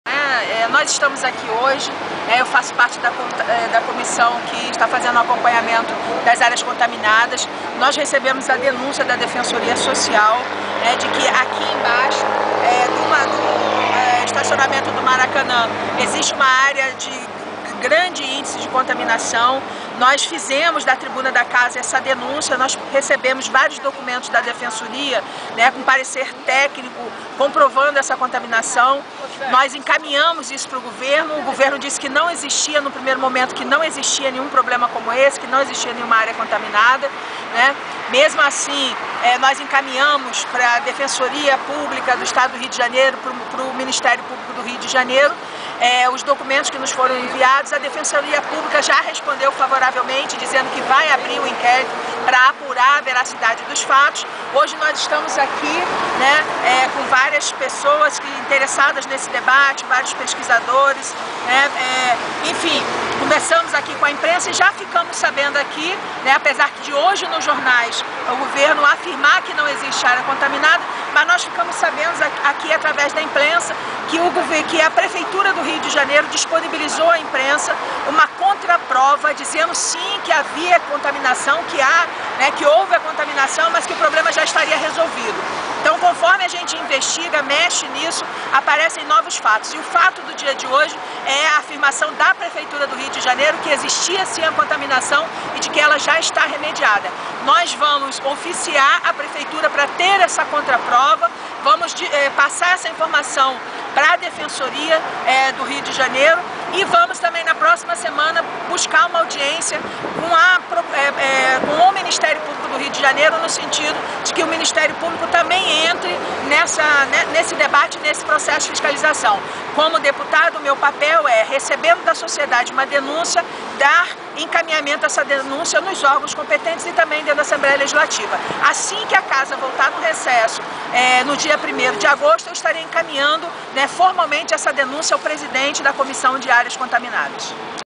Governo Cabral quer esconder contaminação no Maracanã
Deputada Janira Rocha fala sobre área contaminada no estacionamento do Maracanã que governo estadual e Prefeitura do Rio querem esconder.